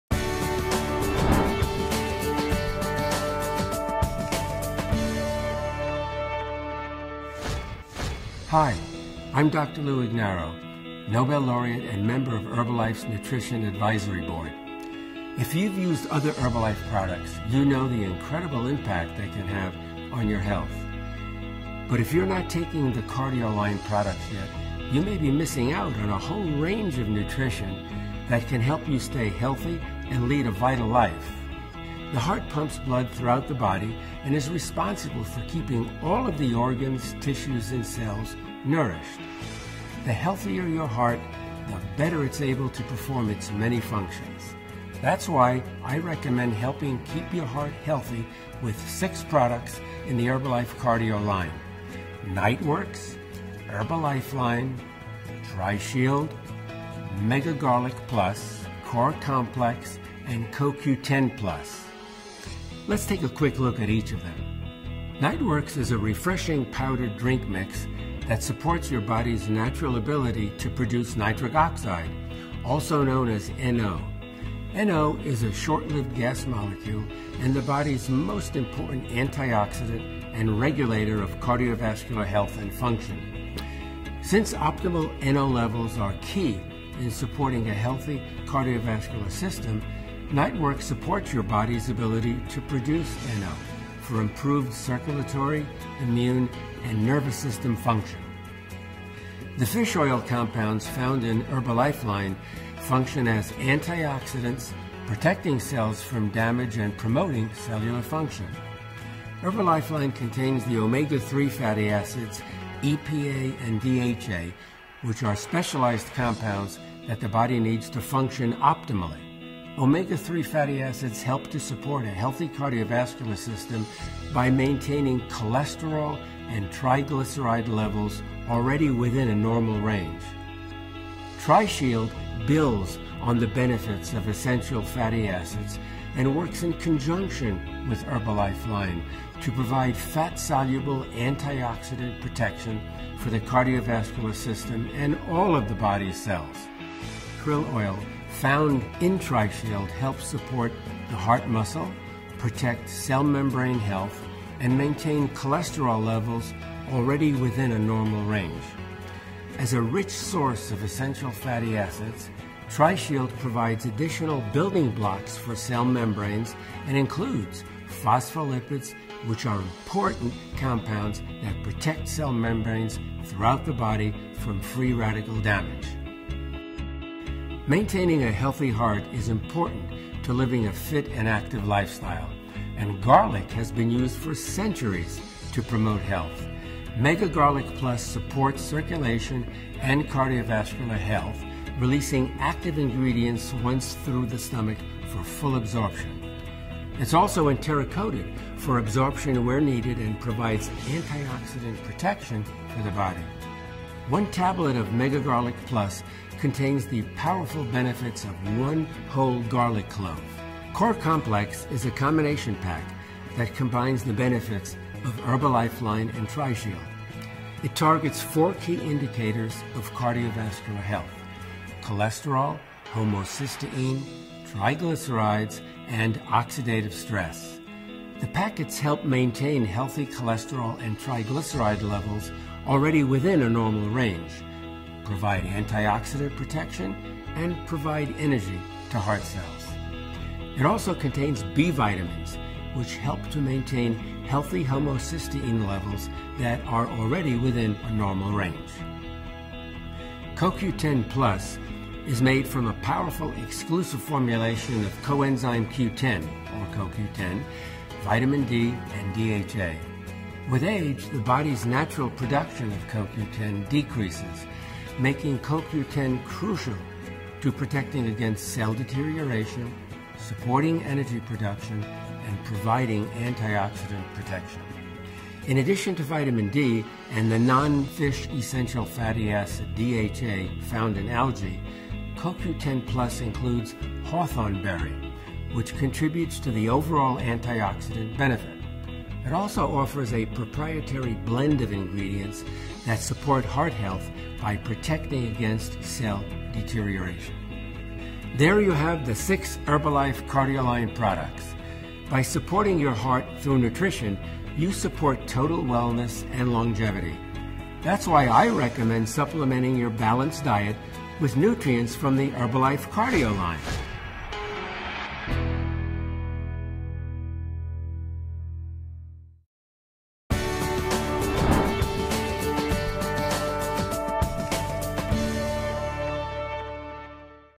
Intro to the Herbalife Cardio Line with Dr. Lou Ignarro
Nobel* laureate and heart expert Dr. Lou Ignarro provides an overview of the six products in the Herbalife cardio line: Niteworks®, Herbalifeline®, Tri-Shield®, Mega Garlic Plus, Core Complex and CoQ10 Plus.
The heart pumps blood throughout the body and is responsible for keeping all of the organs, tissues and cells nourished. The healthier a person’s heart is, the better it’s able to perform its many functions. Dr. Lou Ignarro explains that if you’re not taking the cardio line products yet, you may be missing out on a whole range of nutrition that can support health and vitality.
The cardio line includes:
• Niteworks®, a refreshing powdered drink mix that supports your body’s natural ability to produce Nitric Oxide, also known as NO.
• Herbalifeline®, which contains the Omega-3 fatty acids EPA and DHA, which are specialized compounds that the body needs to function optimally.
• Tri-Shield®, which builds on the benefits of essential fatty acids and works in conjunction with Herbalifeline® to provide fat-soluble antioxidant protection for the cardiovascular system and all of the body’s cells.
• Mega Garlic Plus, which supports circulation and cardiovascular health, releasing active ingredients once through the stomach for full absorption.†
• Core Complex, a combination pack that combines the benefits of Herbalifeline® and Trishield®.
• CoQ10 Plus, which is made from a powerful, exclusive formulation of coenzyme Q10 (CoQ10), vitamin D and DHA.
To learn more about the Herbalife cardio line products, follow this

For more information on how to support heart health, follow this